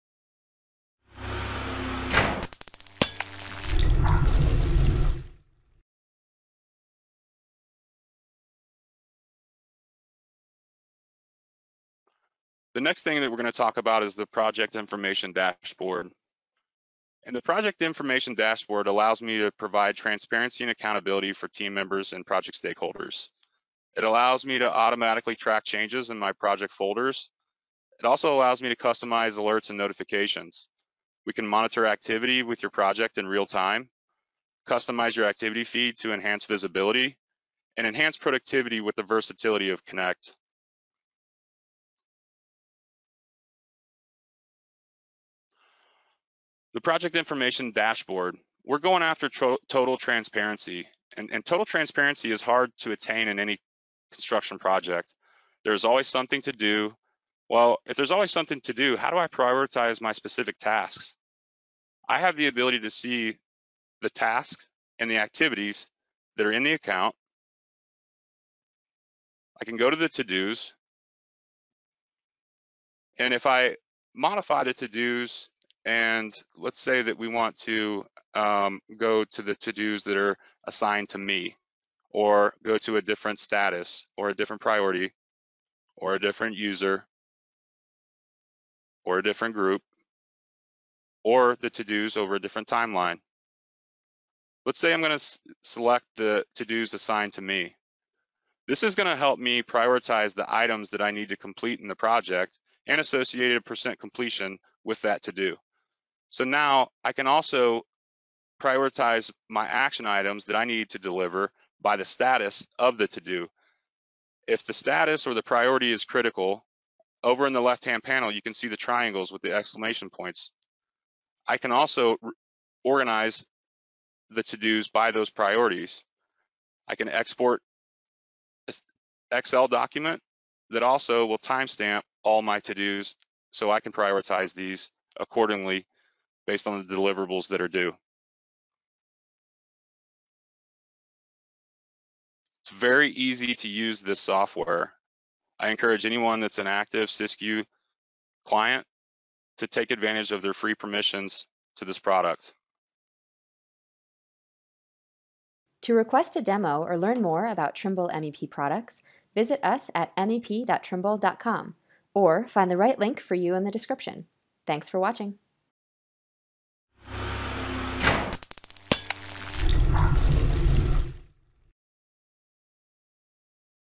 Trimble SysQue and Connect part 6: Product Information Dashboard
Connecting the Right People to the Right Data at the Right Time, learn how Trimble Connect’s integration with SysQue supports collaboration by creating a single source of truth for all your projects. In this video, we will look at  the Project Information Dashboard which will allow you to provide transparency and accountability for team members and project stakeholders.